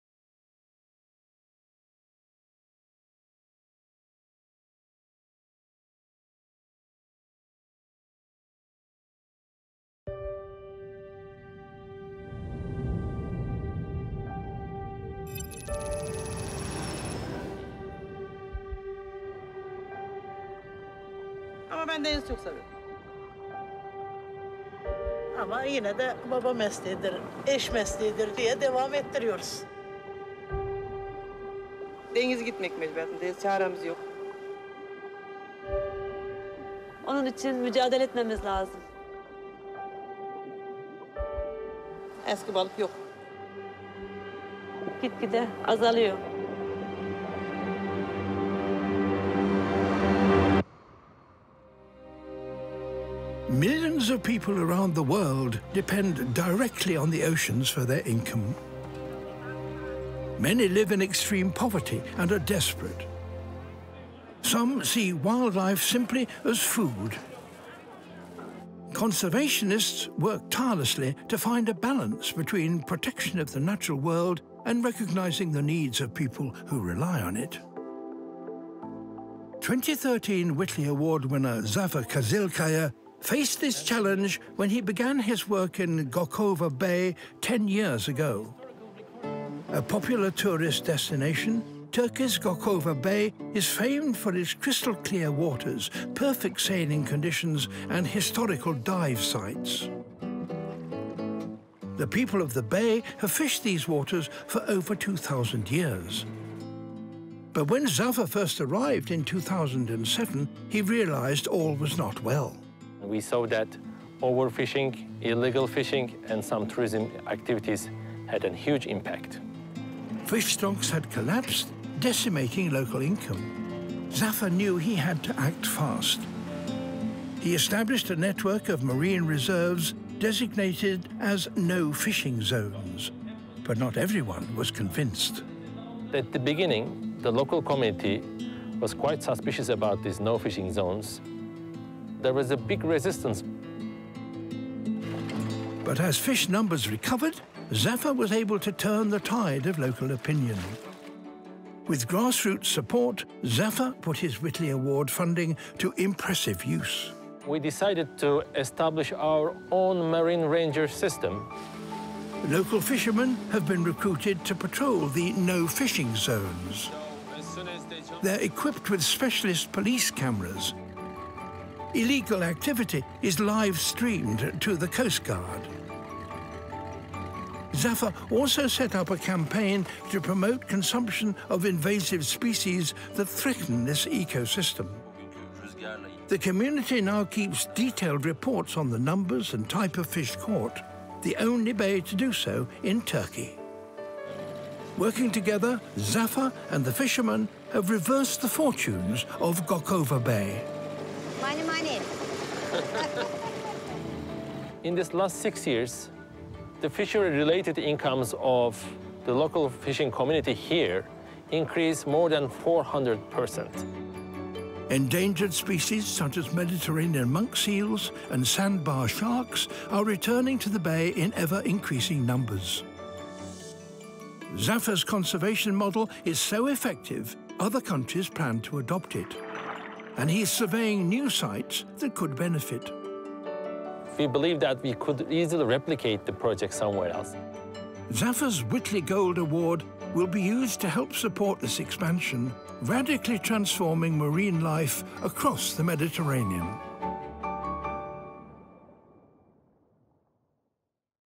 Zafer Kizilkaya, Turkey - Whitley Gold Award 2017
Guardians of the Sea: securing and expanding marine reserves along the Turkish coastline

The Whitley Gold Award donated by The Friends and Scottish Friends of the Whitley Fund for Nature

Thank you to the following people and organisations, this film would not have been possible without your support: